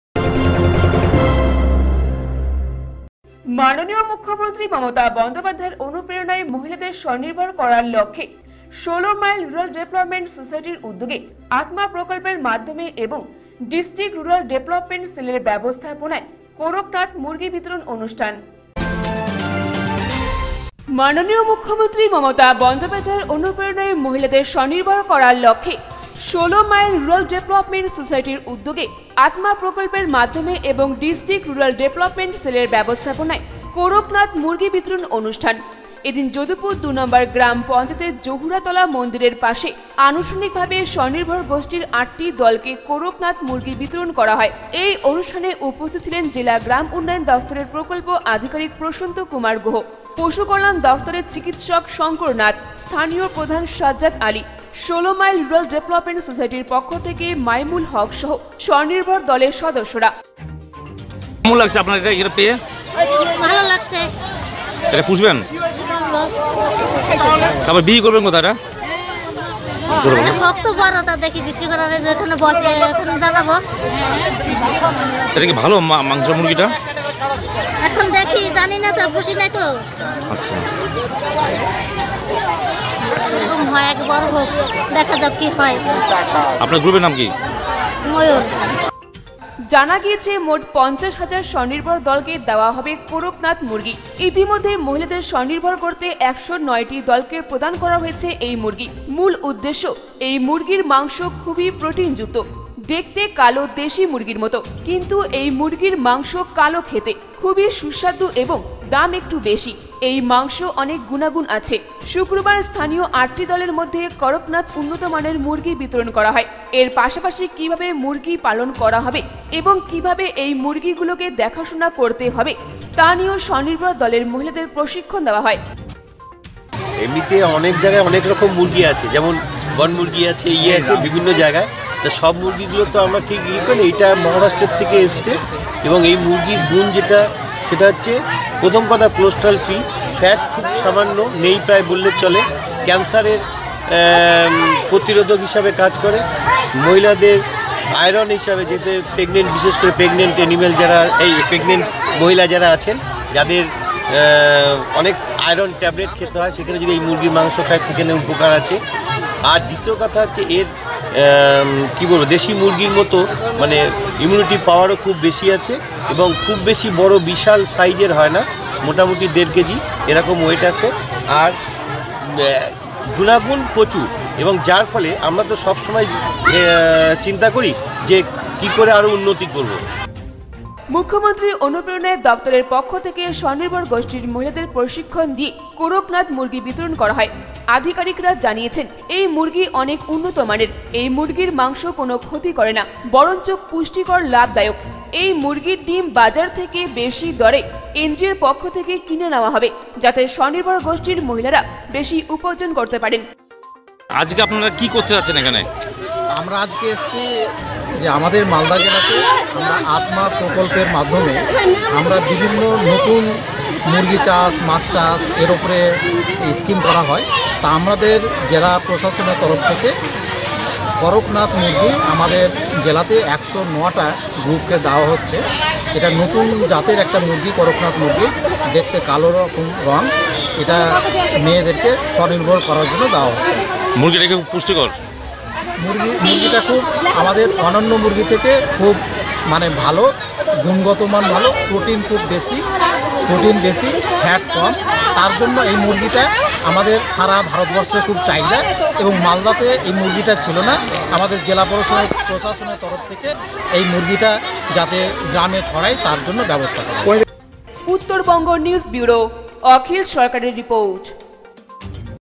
মহিলাদের স্বনির্ভর করার লক্ষ্যে ১৬ মাইল রুরাল ডেভেলপমেন্ট সোসাইটির উদ্যোগে কোরক নাথ মুরগি বিতরণ অনু
মহিলাদের স্বনির্ভর করার লক্ষ্যে ১৬ মাইল রুরাল ডেভেলপমেন্ট সোসাইটির উদ্যোগে  কোরক নাথ মুরগি বিতরণ অনুষ্ঠান